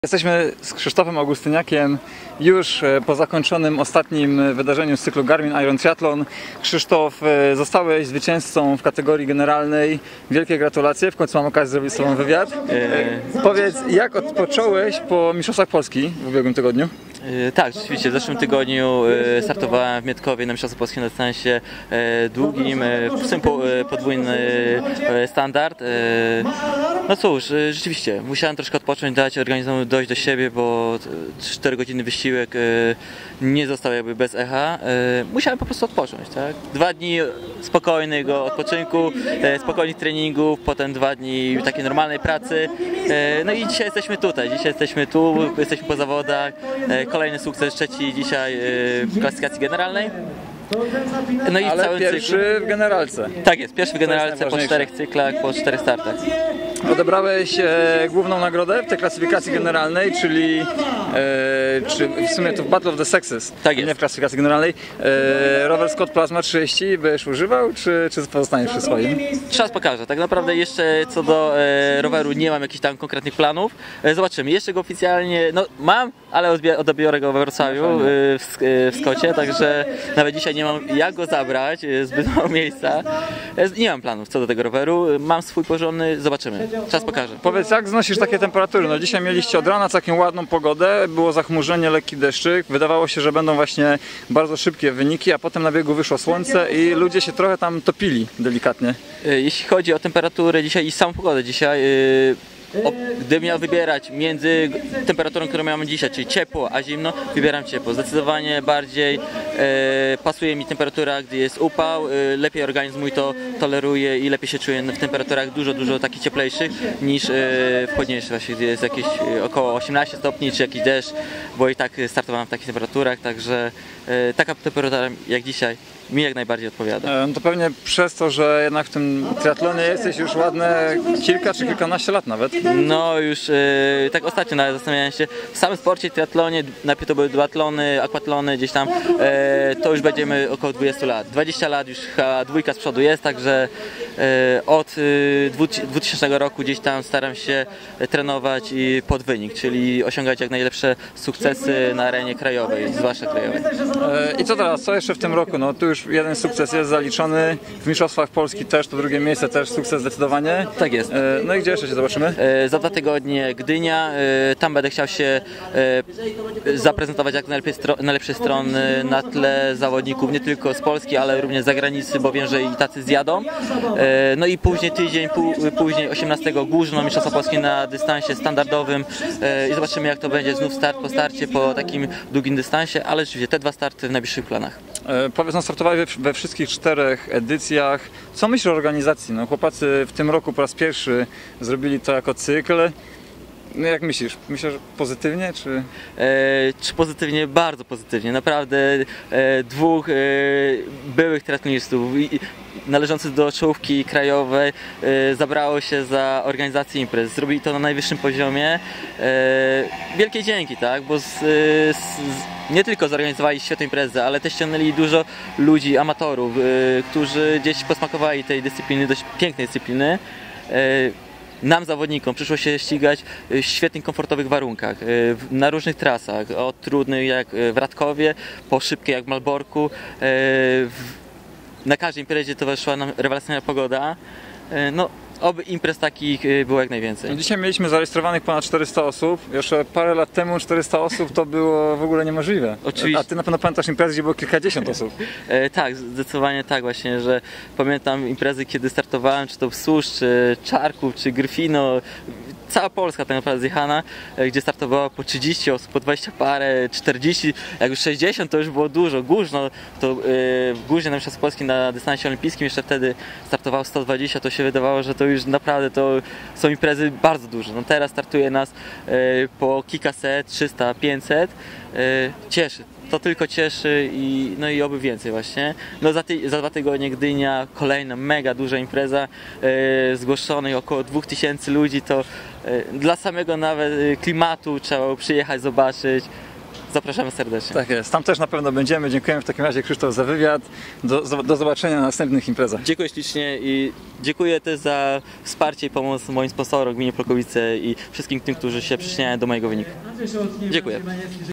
Krzysztof Augustyniak - zwycięzca cyklu Garmin Iron Triathlon- [xTRI.pl]
Paweł Bondaruk rozmawia z Krzysztofem Augustyniakiem - zwycięzcą cyklu Garmin Iron Triathlon.